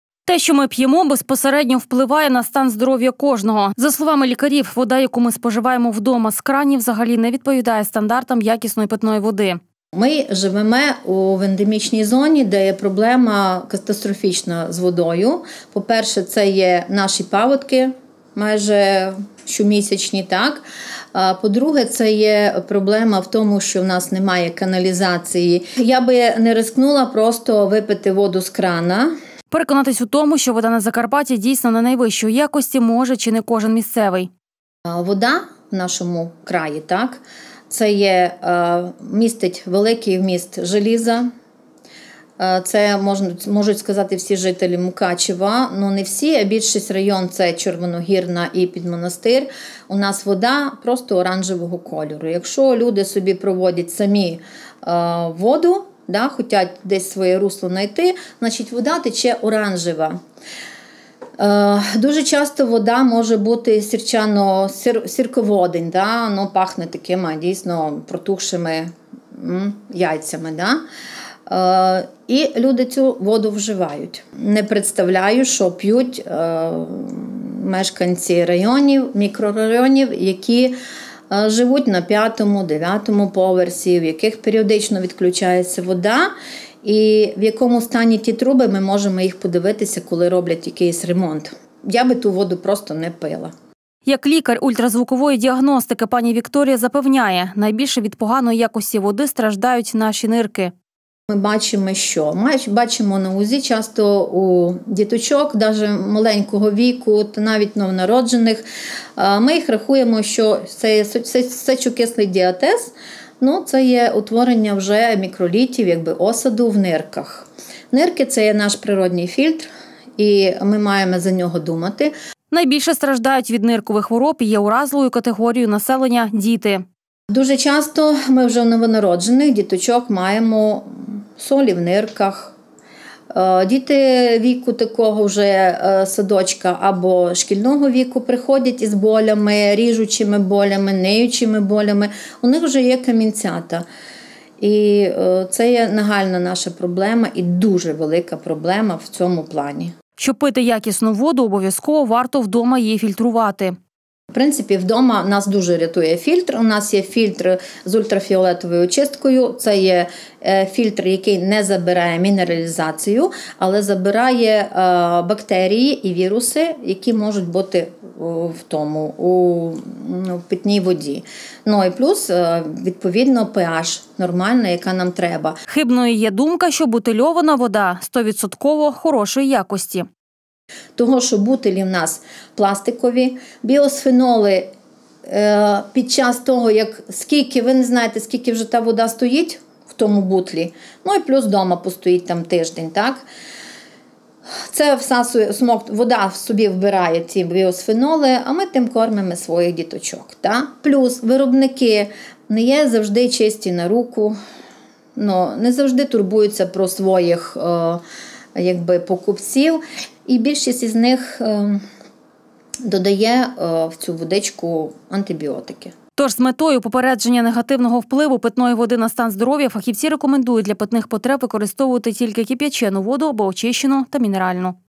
Якість питної води. Наслідки вживання неякісної води
Як відомо, стан організму людини залежить від повітря, способу життя, якості продуктів харчування, а також питної води. Забруднена питна вода згубно впливає на здоров’я людини. Мільярди вірусів і бактерій у воді призводять до спалахів епідемій, інфекційних захворювань, а токсичні речовини – до масових отруєнь. Те, що ми п’ємо безпосередньо впливає на стан здоров’я кожного. За словами лікарів, вода, яку ми споживаємо вдома з кранів взагалі не відповідає стандартам якісної питної води. Переконатись у тому, що вода на Закарпатті дійсно не є найвищої якості може чи не кожен місцевий. Як лікар ультразвукової діагностики, пані Вікторія запевняє, найбільше від поганої якості води страждають наші нирки. Найбільше страждають від ниркових хвороб, і є уразливою категорією населення діти. Щоб пити якісну воду, обов’язково варто вдома її фільтрувати. Хибною є думка, що бутильована вода 100% хорошої якості. Тож з метою попередження негативного  впливу питної води на стан здоров'я фахівці рекомендують  для питних потреб використовувати тільки кип'ячену воду або очищену та мінеральну.